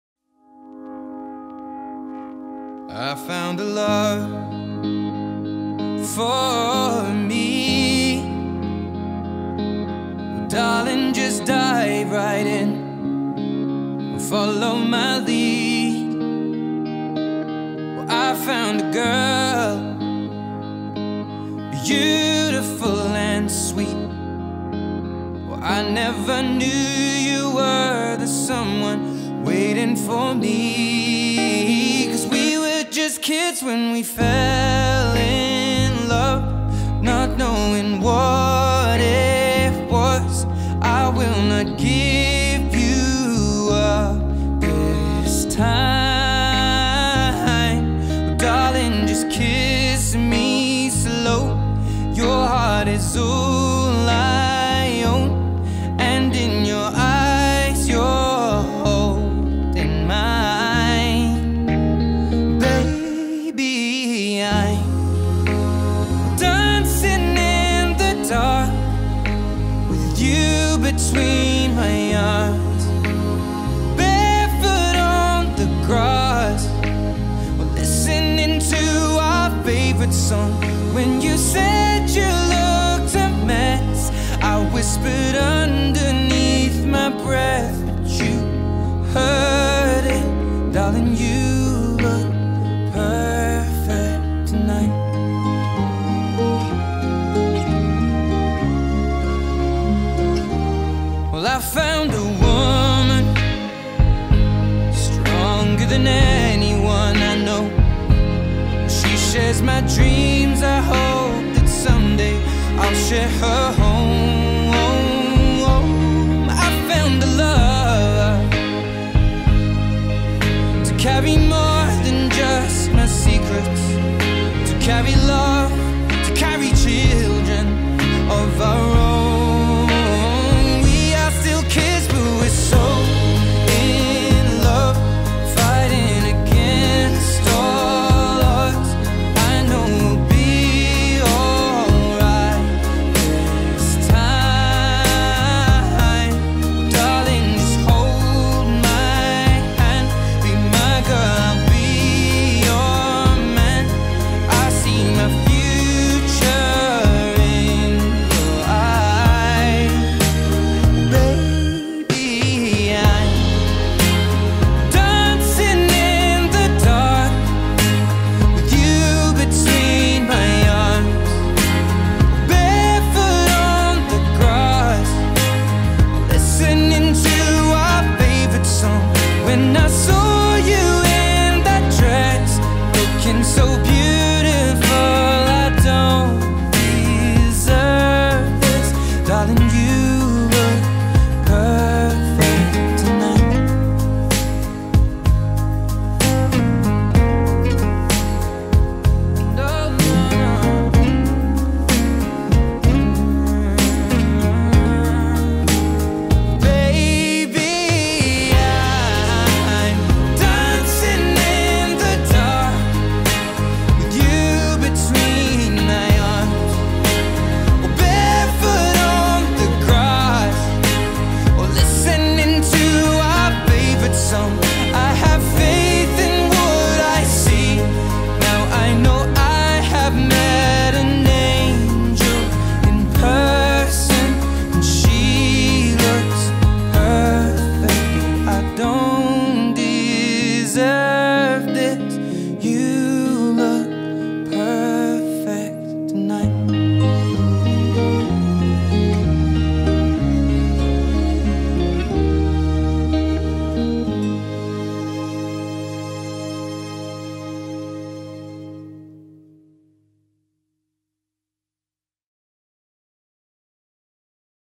Faith and Paul Wedding Highlights at the Slieve Donard Hotel
Highlights of the Big Day from CD Media Productions.